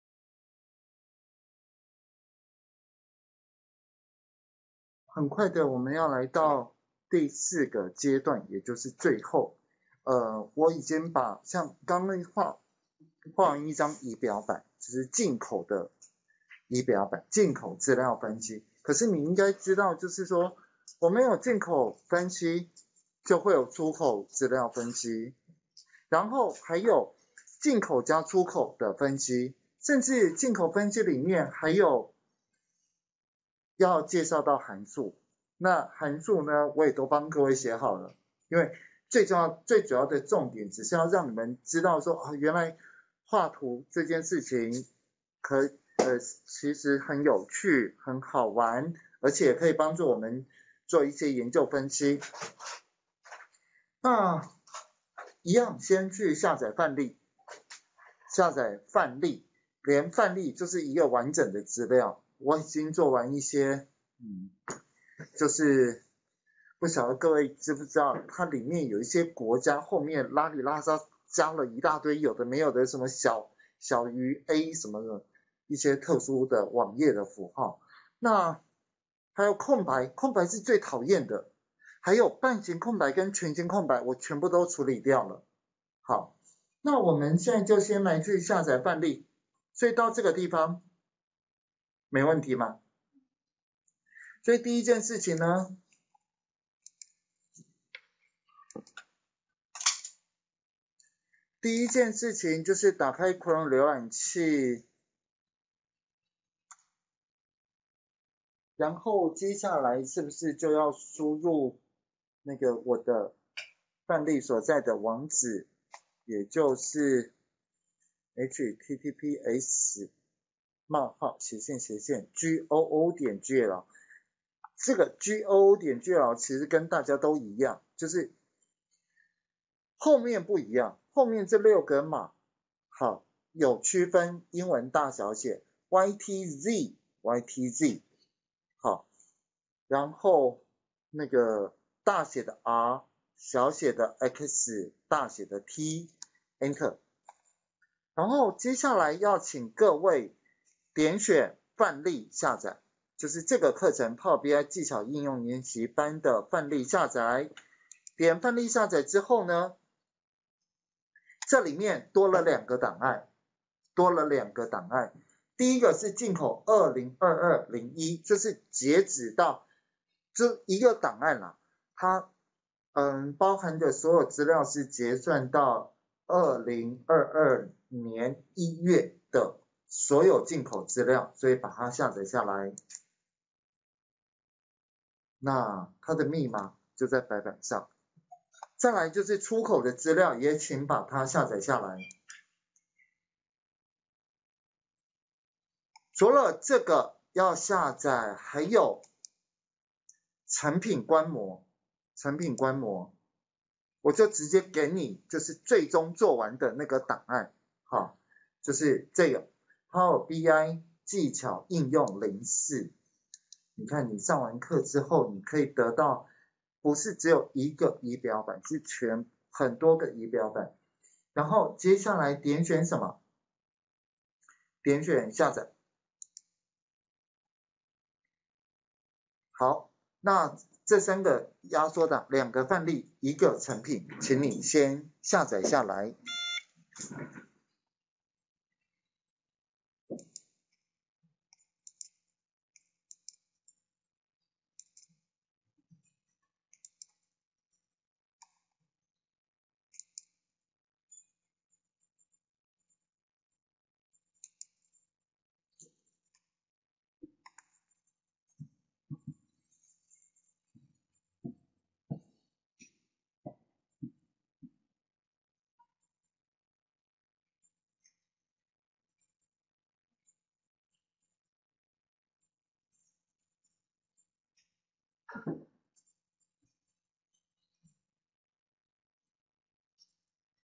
Power BI技巧研習班 67 下載成品範例
下載成品範例
Power BI技巧研習班-07.視覺化分析4。資料分析目的，瞭解資料轉置與樞紐的差別。轉置的使用時機，使用功能操作，不用寫複雜函數，相信人人都可以理解。